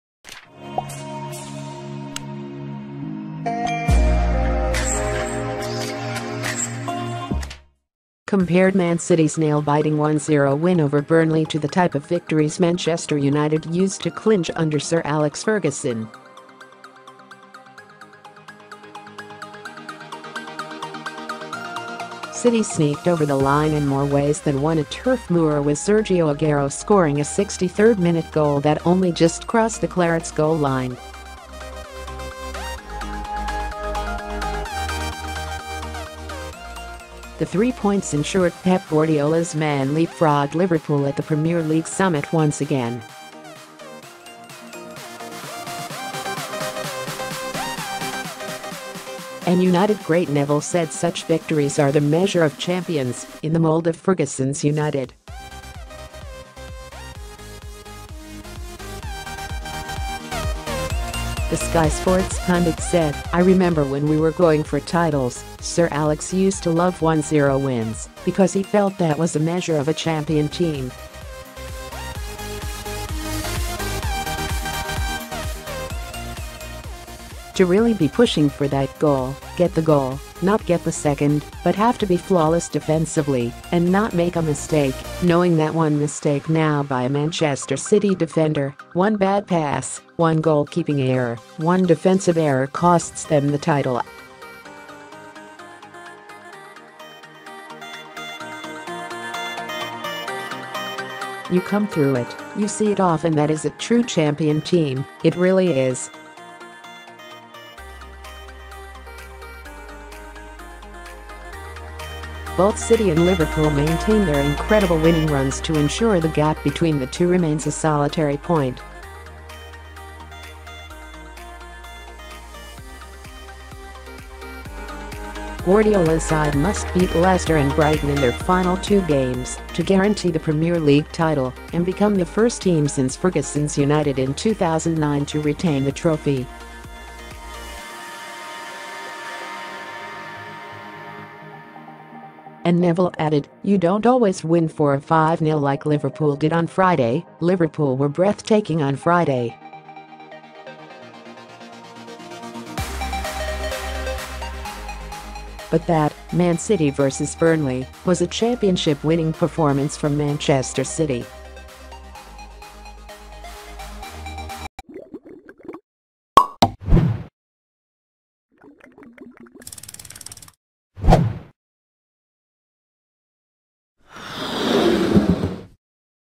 Man City's win over Burnley had Neville recalling Ferguson's Man Utd Man City News:
compared Man City's nail-biting 1-0 win over Burnley to the type of victories Manchester United used to clinch under Sir Alex Ferguson.   City sneaked over the line in more ways than one at Turf Moor with Sergio Aguero scoring a 63rd minute goal that only just crossed the Clarets' goal-line. The three points ensured Pep Guardiola's men leapfrogged Liverpool at the Premier League's summit once again.   And United great Neville said such victories are the "measure" of champions — in the mould of Ferguson's United.  The Sky Sports pundit said: "I remember when we were going for titles, Sir Alex used to love 1-0 wins because he felt that was a measure of a champion team.     “To really be pushing for that goal, get the goal — not get the second but have to be flawless defensively and not make a mistake, knowing that one mistake now by a Manchester City defender, one bad pass, one goalkeeping error, one defensive error costs them the title.  "You come through it, you see it off and that is a true champion team, it really is."  Both City and Liverpool maintained their incredible winning runs to ensure the gap between the two remains a solitary point.  Guardiola's side must beat Leicester and Brighton in their final two games to guarantee the Premier League title and become the first team since Ferguson's United in 2009 to retain the trophy.   And Neville added: “You don’t always win four of five nil like Liverpool did on Friday — Liverpool were breathtaking on Friday.  “But that (Man City vs Burnley) was a championship-winning performance from Manchester City.”